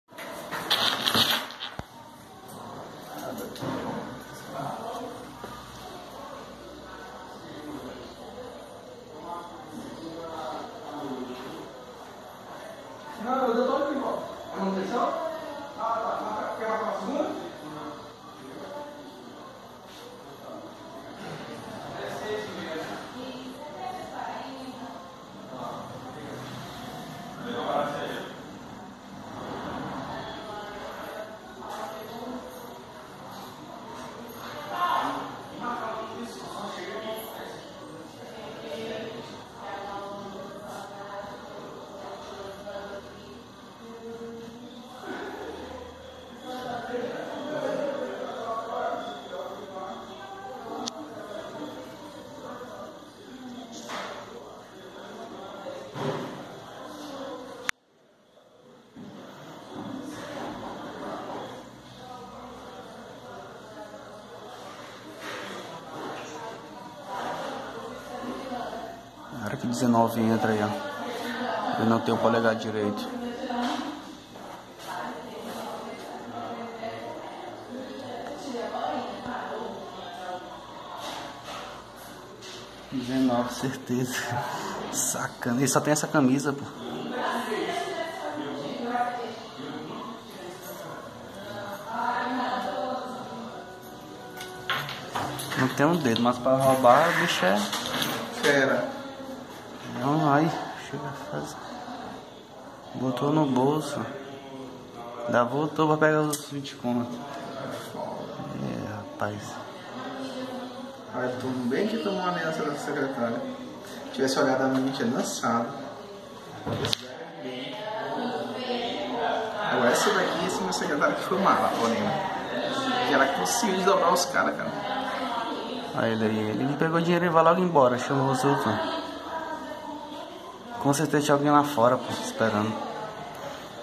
Clínica odontológica é assaltada no Anjo da Guarda
Policiais do 5º DP do bairro Anjo da Guarda, em São Luís, deram cumprimento a mandado de prisão preventiva contra Laércio Sá Marques.

Pelas informações da polícia, na quarta-feira (6), o assaltante, em companhia de dois comparsas, invadiram e roubaram o Restaurante Santa Ceia, no Bacanga, levando pertences de funcionários e clientes.